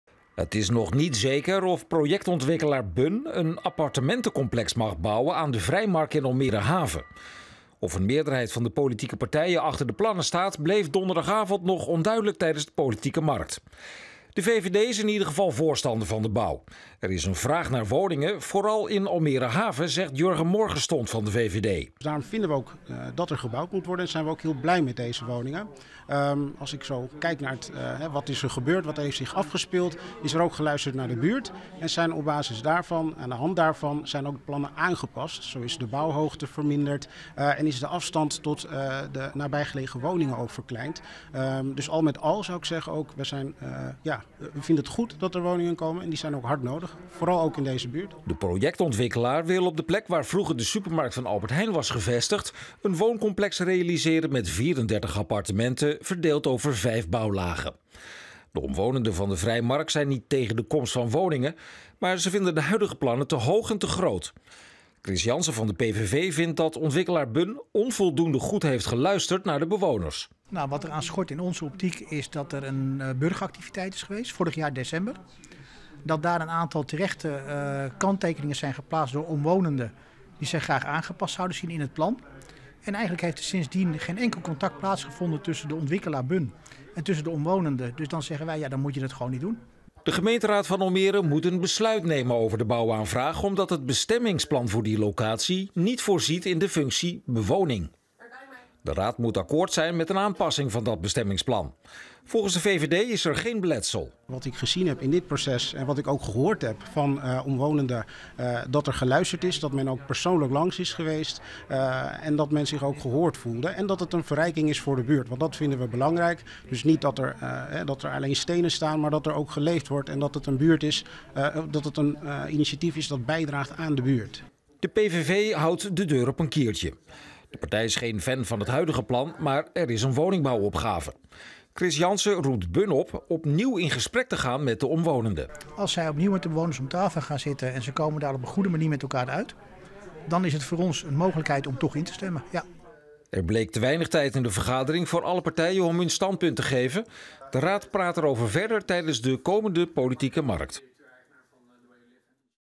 Komst appartementencomplex Vrijmark nog niet zeker
Het is nog niet zeker of projectontwikkelaar BUN een appartementencomplex mag bouwen aan de Vrijmark in Almere Haven. Of een meerderheid van de politieke partijen achter die plannen staat, bleef donderdagavond nog onduidelijk tijdens de Politieke Markt.

De projectontwikkelaar wil op de plek waar vroeger de supermarkt van Albert Heijn gevestigd was een wooncomplex realiseren met 34 appartementen verdeeld over 5 bouwlagen. De omwonenden van de Vrijmark zijn het daar niet mee eens. Zij vrezen een aantasting van hun privacy omdat het geplande gebouw hoger wordt dan de naastgelegen woningen. Daarnaast maken de bewoners zich zorgen over de toename van verkeersdruk en lawaai.